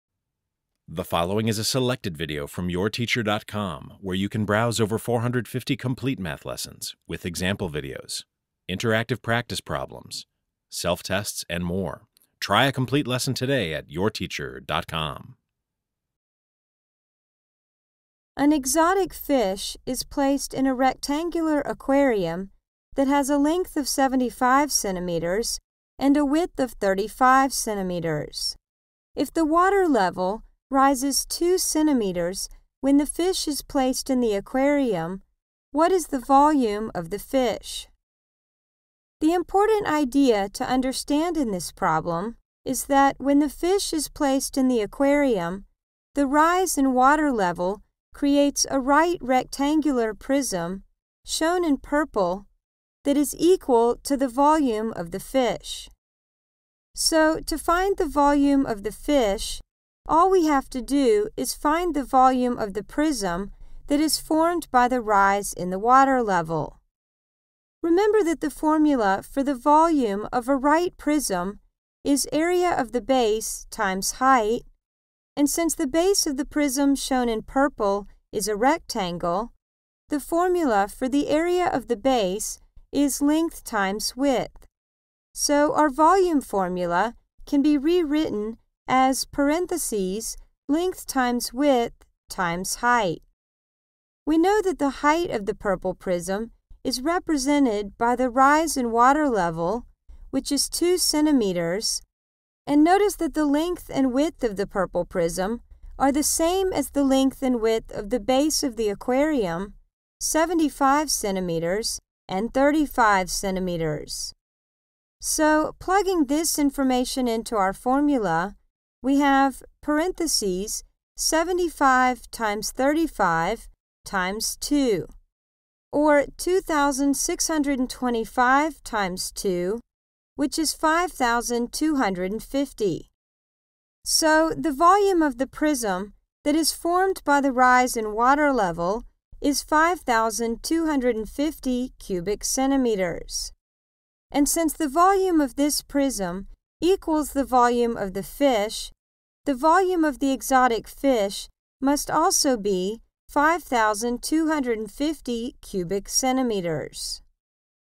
Volume Word Problems - MathHelp.com - Geometry Help
For a complete lesson on volume word problems, go - 1000+ online math lessons featuring a personal math teacher inside every lesson! In this lesson, students learn the definition of a prism, as well as the following vocabulary words related to prisms: base, base edge, lateral edge, lateral face, and altitude (height). Students also learn the formulas for the lateral area, total area, and volume of a right prism, and are asked to solve problems using these formulas. Note that the prisms in this lesson are primarily rectangular prisms -- prisms whose bases are rectangles.